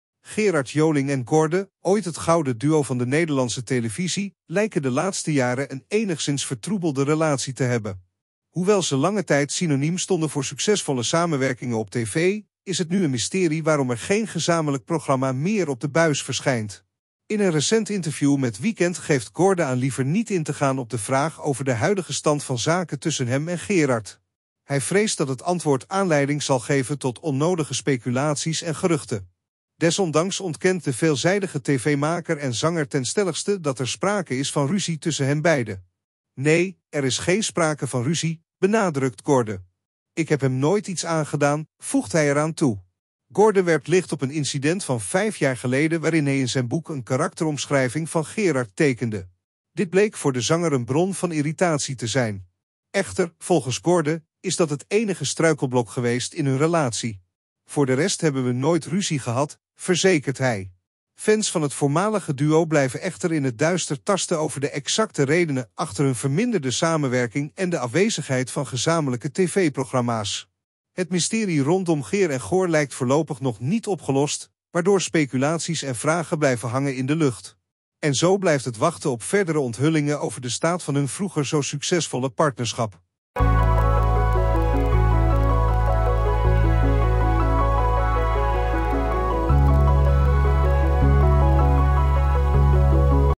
Gordon onthult openhartig over relatie met Gerard Joling!
Gerard Joling en Gordon, ooit het gouden duo van de Nederlandse televisie, lijken de laatste jaren een enigszins vertroebelde relatie te hebben. Hoewel ze lange tijd synoniem stonden voor succesvolle samenwerkingen op tv, is het nu een mysterie waarom er geen gezamenlijk programma meer op de buis verschijnt.

In een recent interview met Weekend geeft Gordon aan liever niet in te gaan op de vraag over de huidige stand van zaken tussen hem en Gerard. Hij vreest dat het antwoord aanleiding zal geven tot onnodige speculaties en geruchten. Desondanks ontkent de veelzijdige tv-maker en zanger ten stelligste dat er sprake is van ruzie tussen hen beiden. "Nee, er is geen sprake van ruzie," benadrukt Gordon. "Ik heb hem nooit iets aangedaan," voegt hij eraan toe.

Gordon werpt licht op een incident van vijf jaar geleden waarin hij in zijn boek een karakteromschrijving van Gerard tekende. Dit bleek voor de zanger een bron van irritatie te zijn. Echter, volgens Gordon, is dat het enige struikelblok geweest in hun relatie. "Voor de rest hebben we nooit ruzie gehad," verzekert hij.

Fans van het voormalige duo blijven echter in het duister tasten over de exacte redenen achter hun verminderde samenwerking en de afwezigheid van gezamenlijke tv-programma's. Het mysterie rondom Geer en Goor lijkt voorlopig nog niet opgelost, waardoor speculaties en vragen blijven hangen in de lucht. En zo blijft het wachten op verdere onthullingen over de staat van hun vroeger zo succesvolle partnerschap.